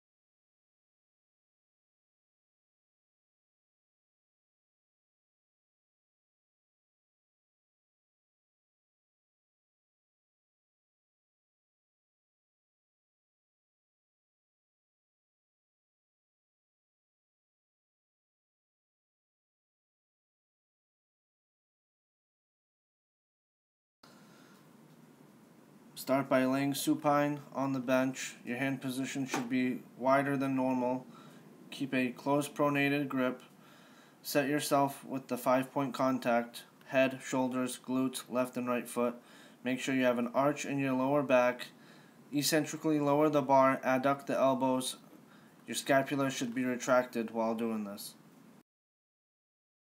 Powerlifting Bench Press W/ Chains
1. Lay supine on the bench
2. Ensure you have the 5 point contact ( Head, shoulders, buttocks, and both feet on the floor)
3. Make sure chains are rest on the floor
4. A closed pronated grip is required
5. Retract your scapula & push your chest out
6. Make sure you have an arch in your lower back
7. On the eccentric phase adduct the elbows
8. The bar needs to touch the chest (no bouncing the bar)
9. Both arms need to extend  at the same time on the concentric phase